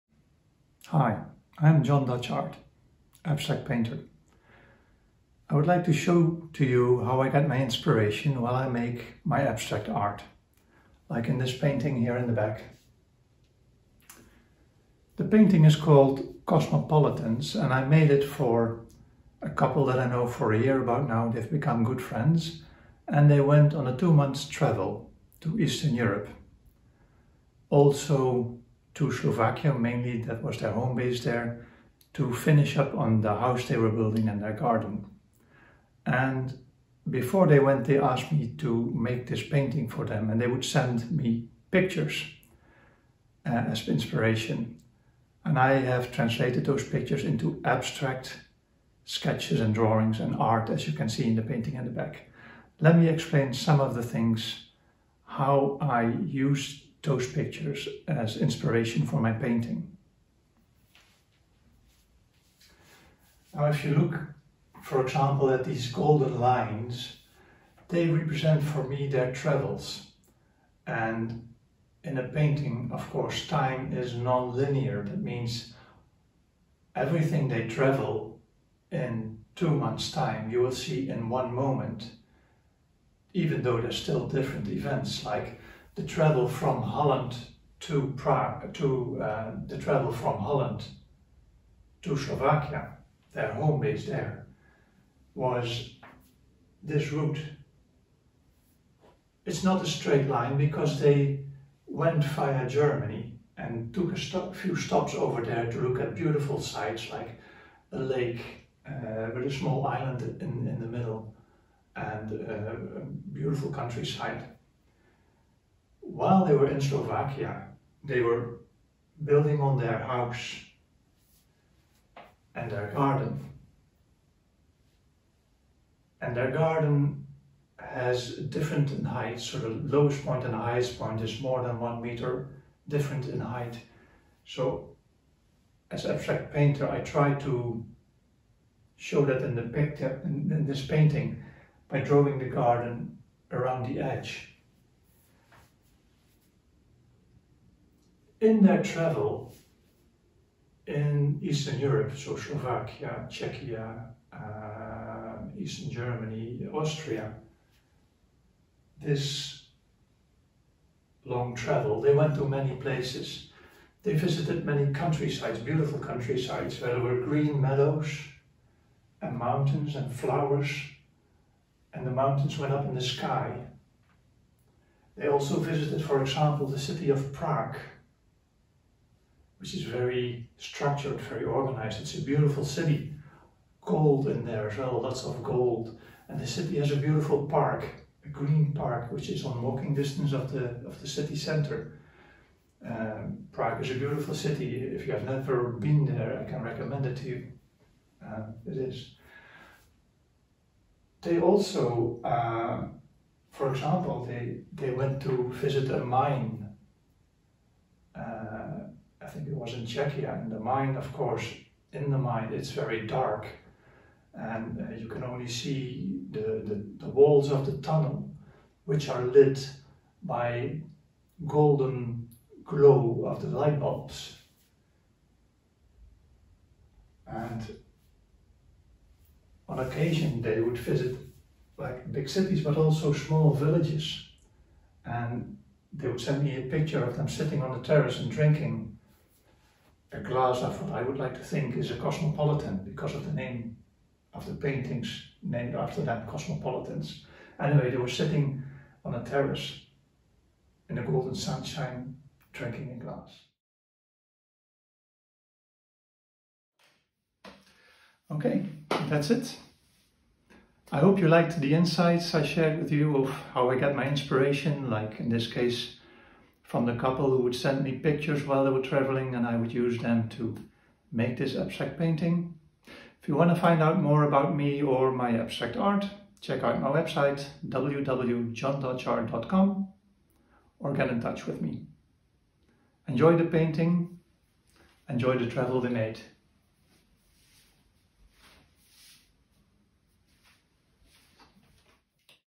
What inspires an abstract painter when creating an artwork
In this video I explain how I, as an abstract painter, get my inspiration when creating a painting. I illustrate how I got inspired for a painting and how I abstracted this to abstract art.
I received a commission for this painting by a couple that asked me to create a painting for their living room, while they were travelling for 2 months.

I wanted to create an abstract painting that would be very personal to them. But Initially I did not find the inspiration.
 ... until I saw their posts on social media.